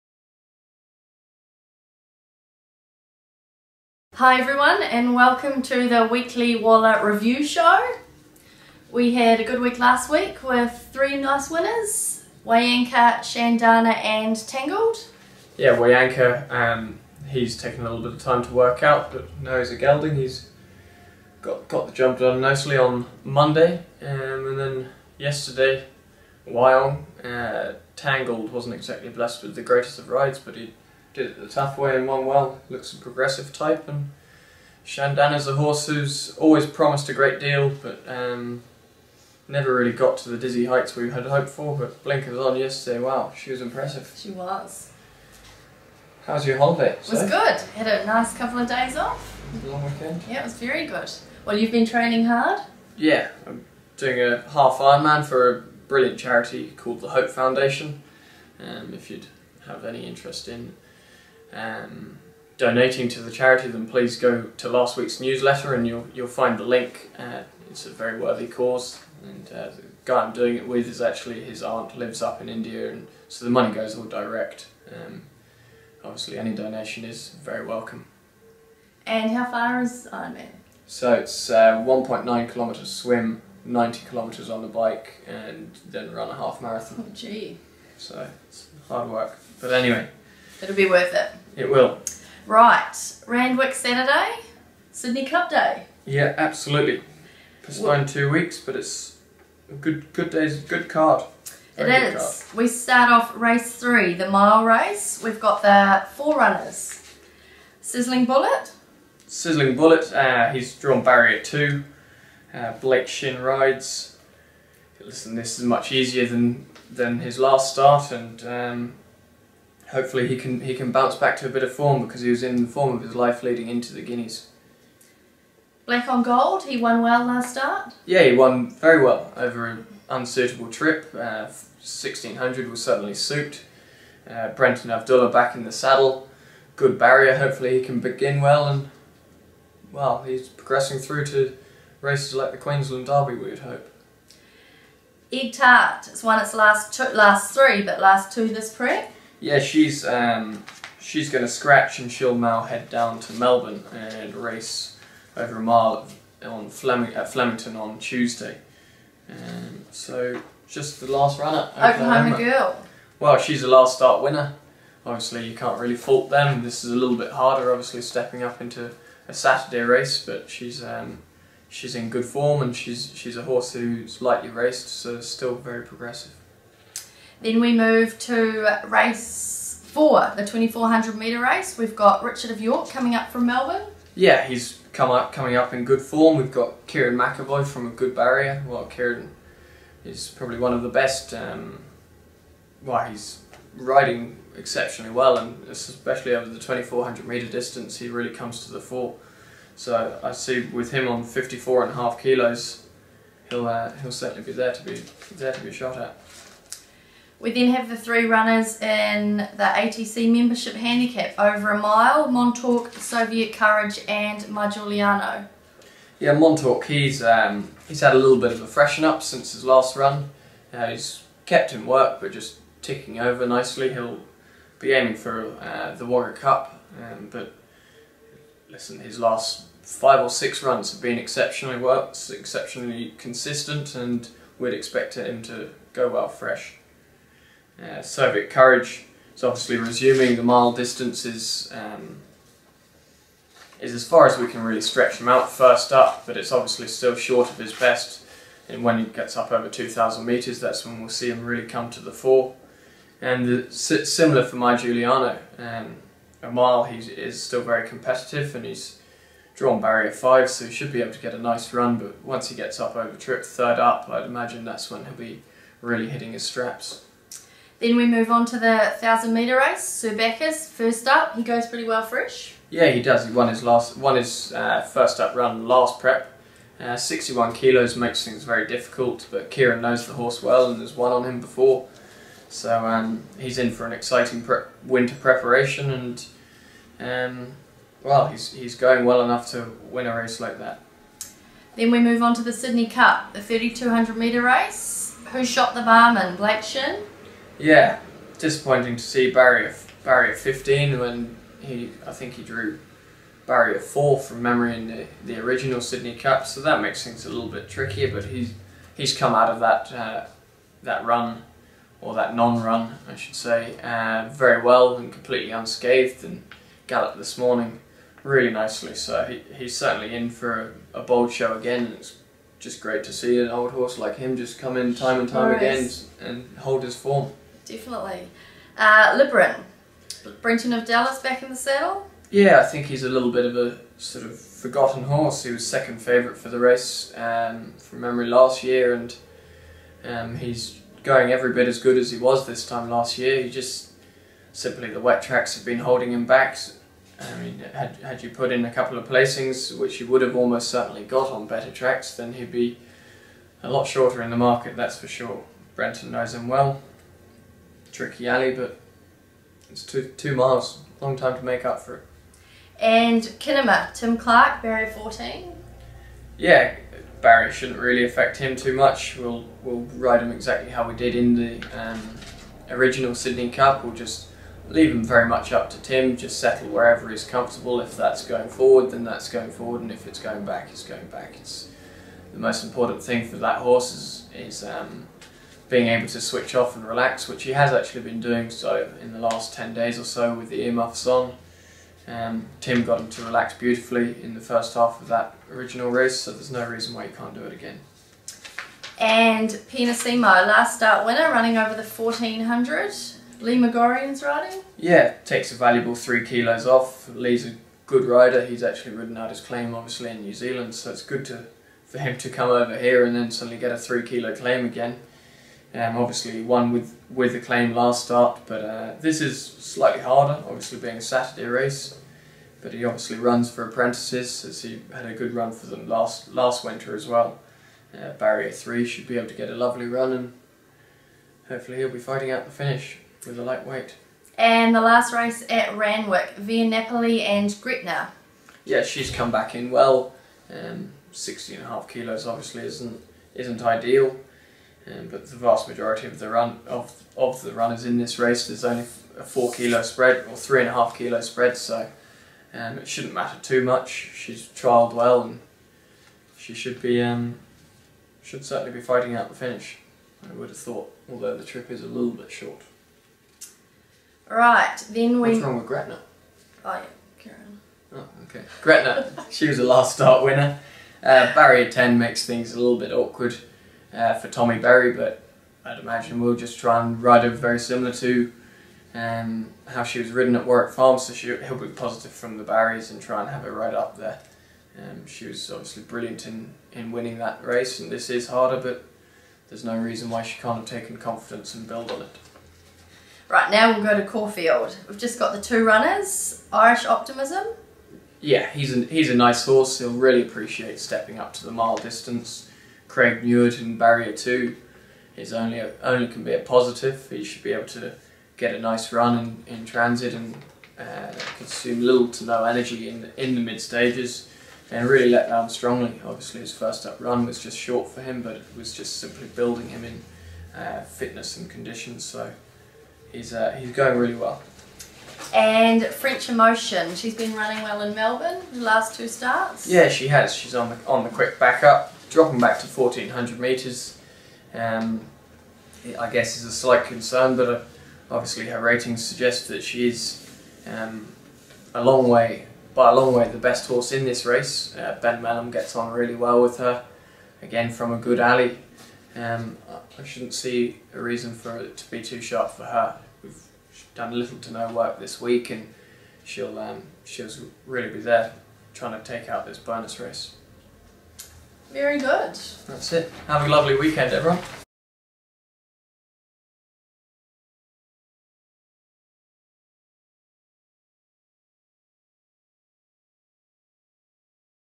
Chris Waller Racing Runners Preview - 22nd April 2017
Charlie and Sophie discuss the past week for the stable, as well as the upcoming runners for the weekend.